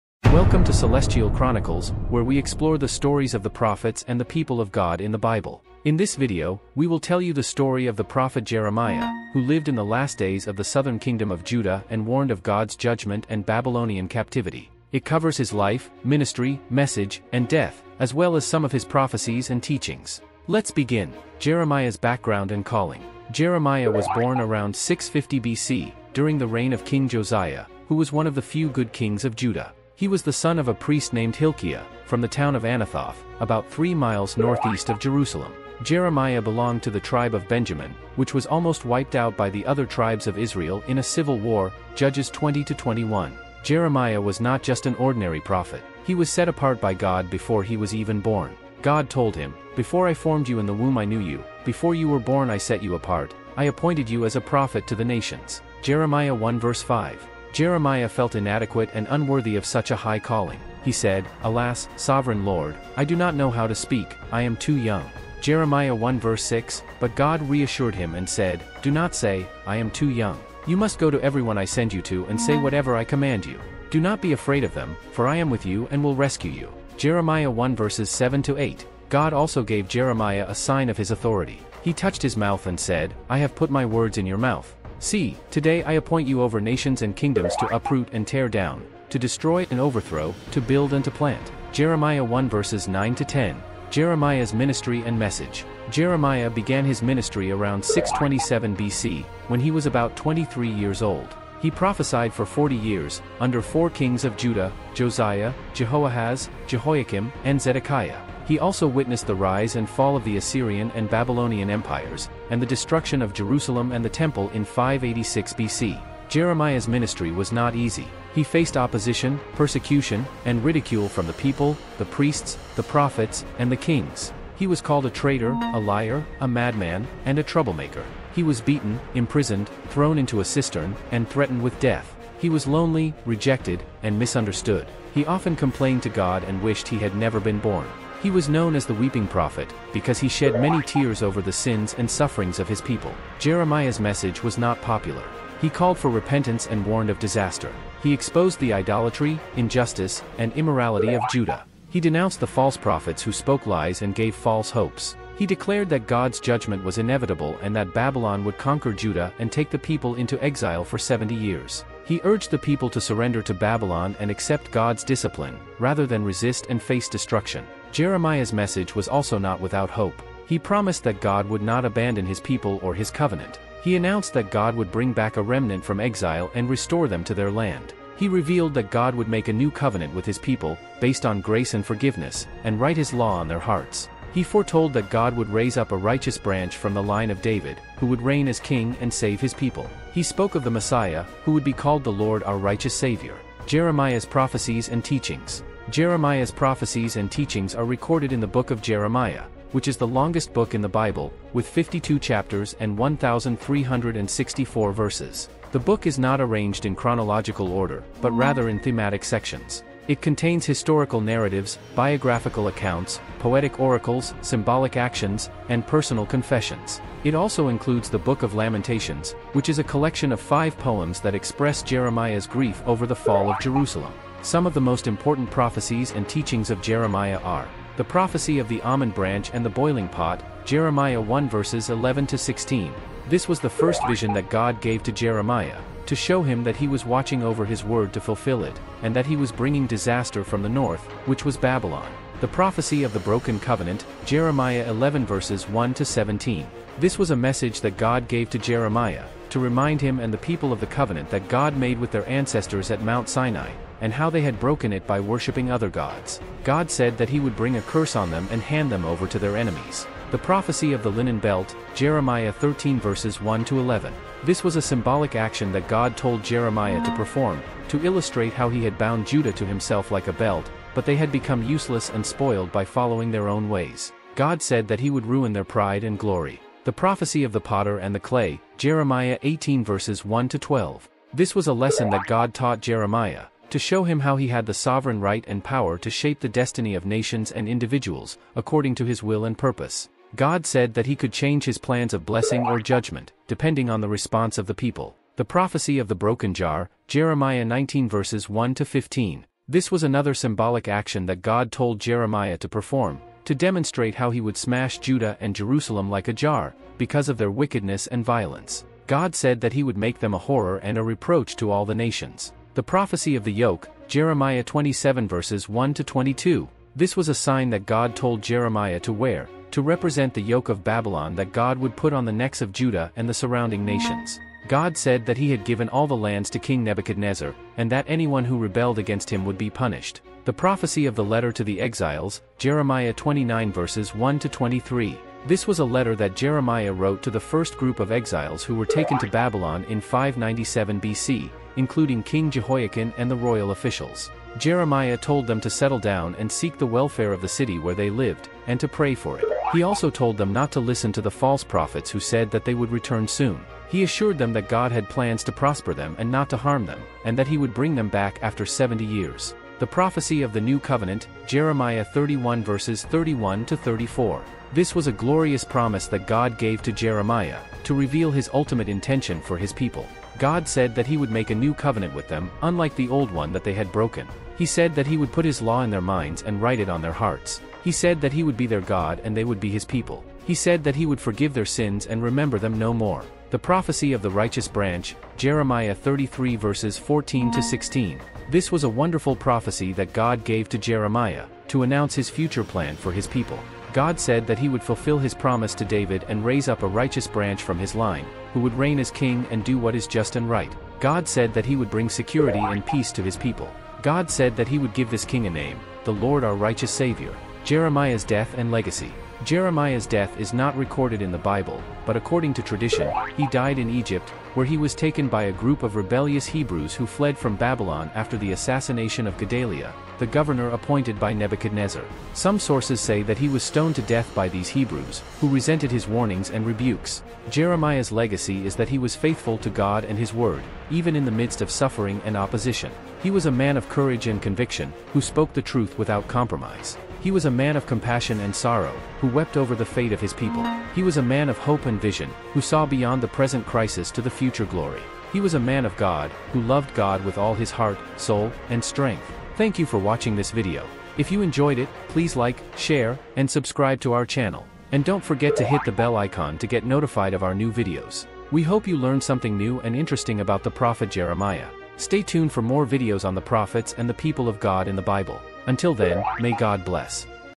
The Story of Jeremiah A Prophet’s Tale
ake a journey back in time and meet Jeremiah, a prophet with a divine hotline. From his calling to his prophecies, explore the life of this weeping prophet and man of faith.